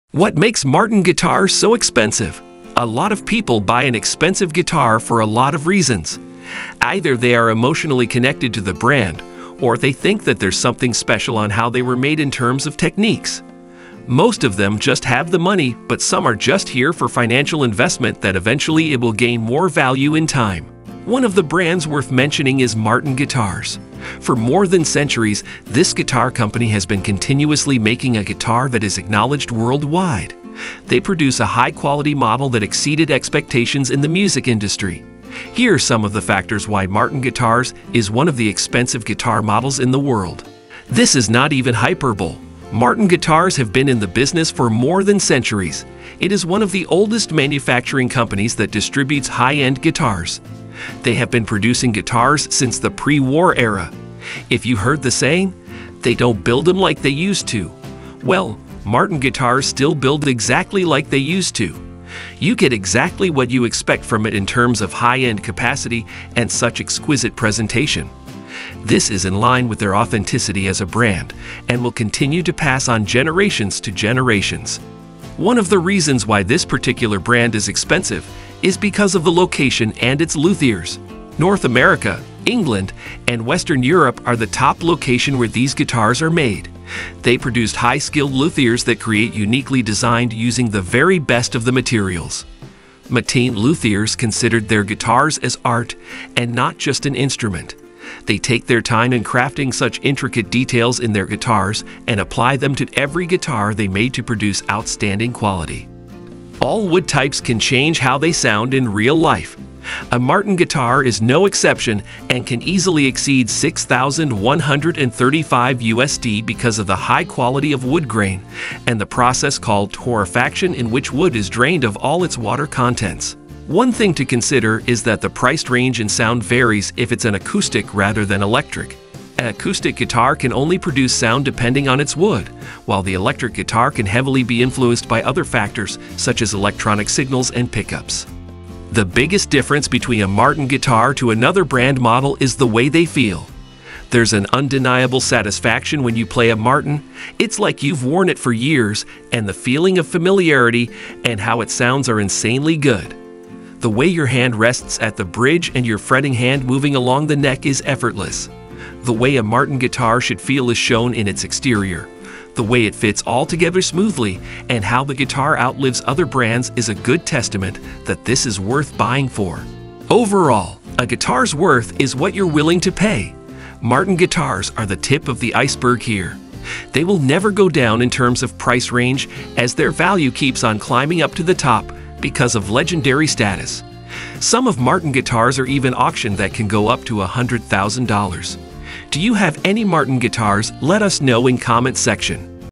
Martin Guitar - Why You Should Have a Martin Guitar? Worth The Money?
While CF Martin is an American company, their roots are in Germany. Christian Friedrich Martin was originally a German guitar maker, but his competitors worried the guitar would overtake the violin. They attempted to have him banned from guitar manufacturing in the country. Thankfully, he was able to beat the court cases and continue making guitars. However, he needed somewhere to make guitars freely, without the constant threat of being shut down by jealous competitors.

Watch this video to know more about martin guitars  Why You Should Have a Martin Guitar? is it Worth The Money?